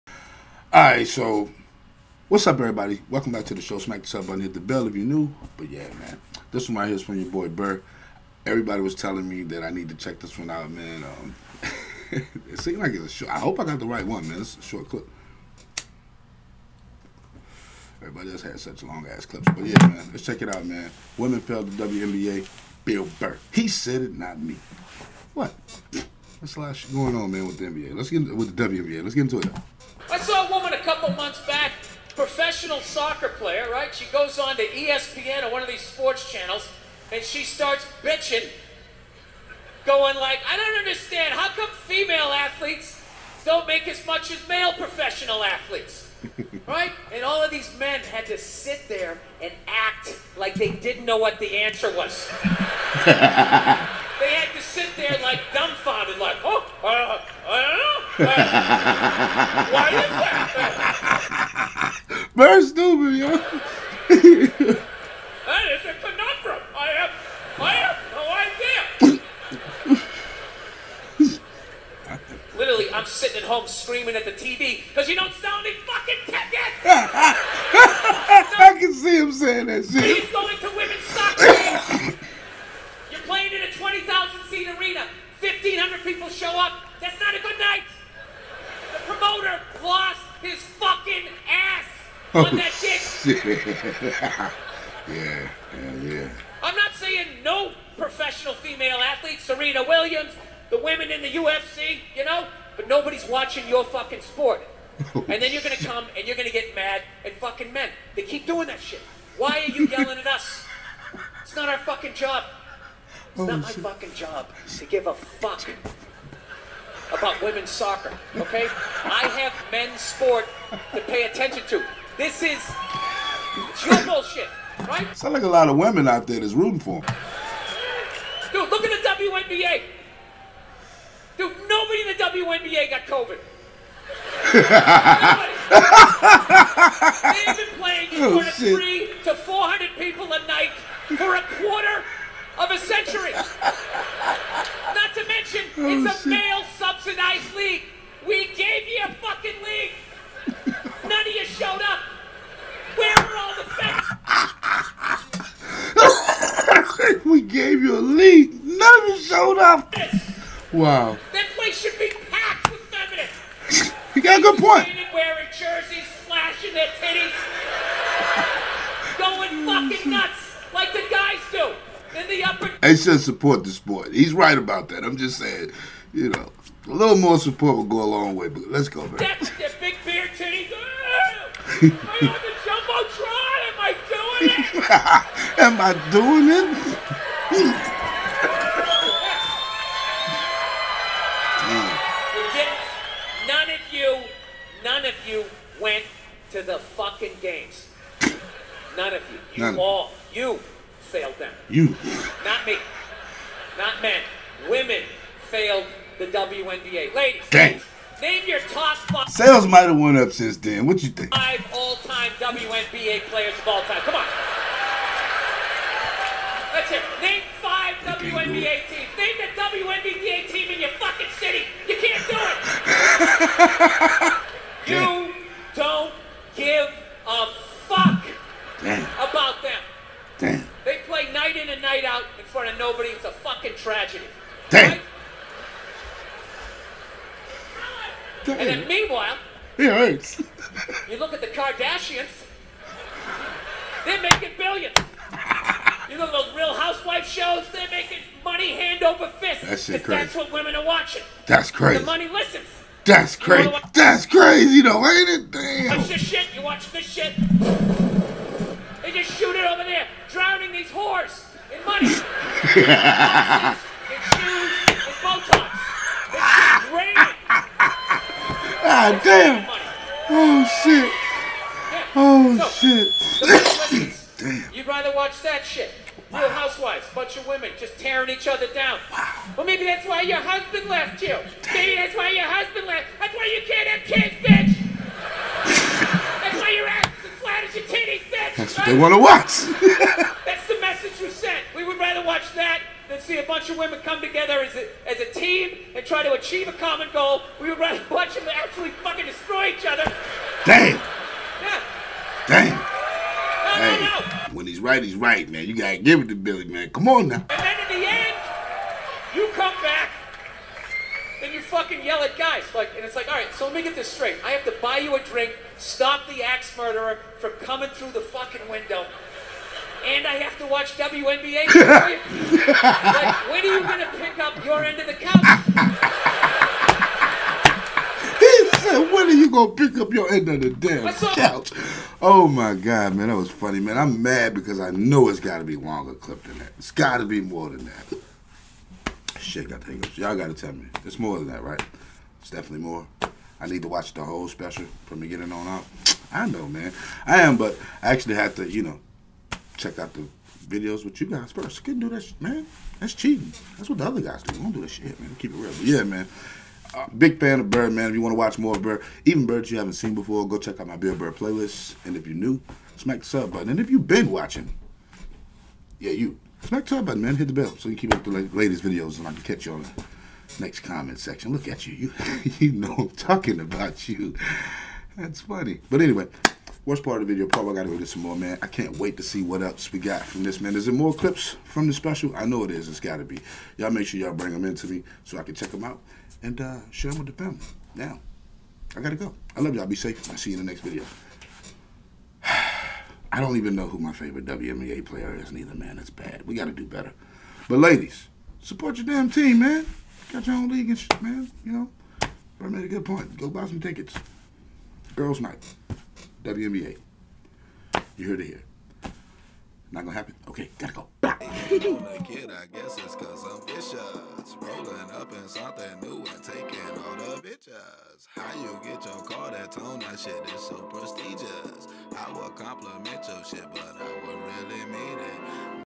BILL BURR PREDICTED ANGEL REESE & CAITLIN CLARK??
THIS IS A REACTION THAT SOMEONE IN THE FAMILY WANTED US TO SHARE PLEASE ENJOY